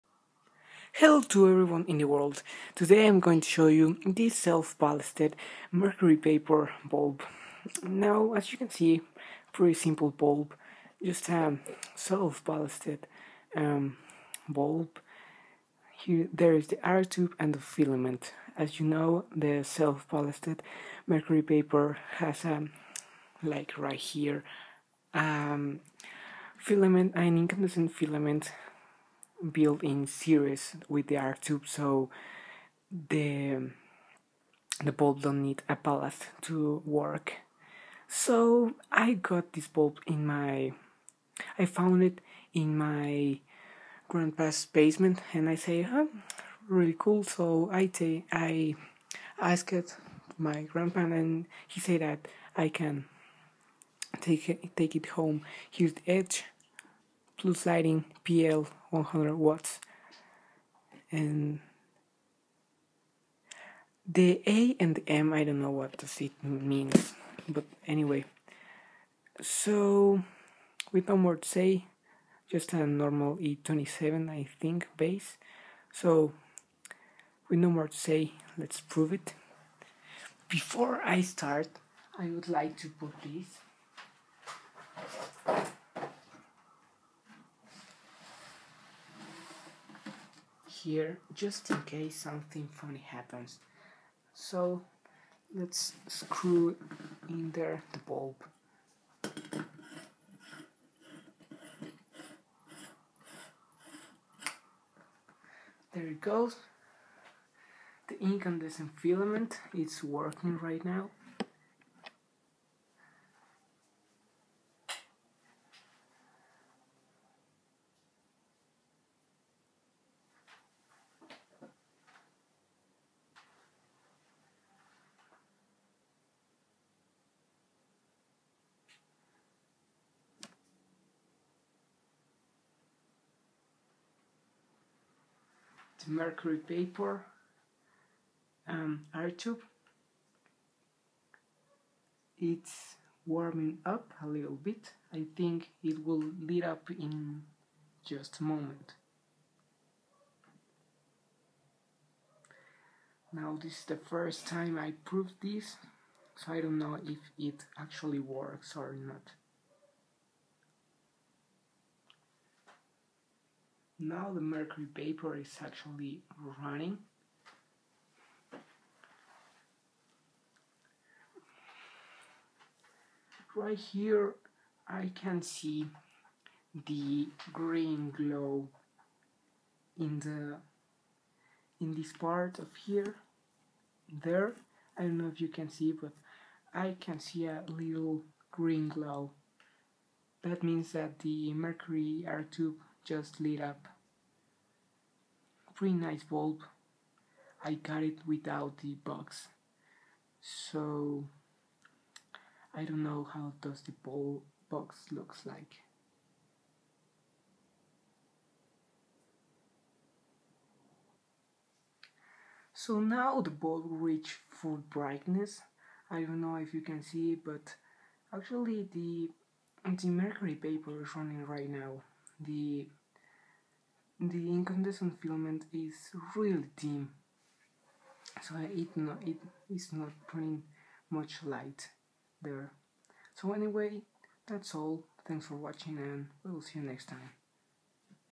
Self-Ballasted 100W Mercury Vapor Lamp.
My first self-ballasted mercury vapor lamp! This bulb has an small arc tube built in series with an incandescent tungsten filament that works like a ballast for the bulb. This one is 100W and works with 120V.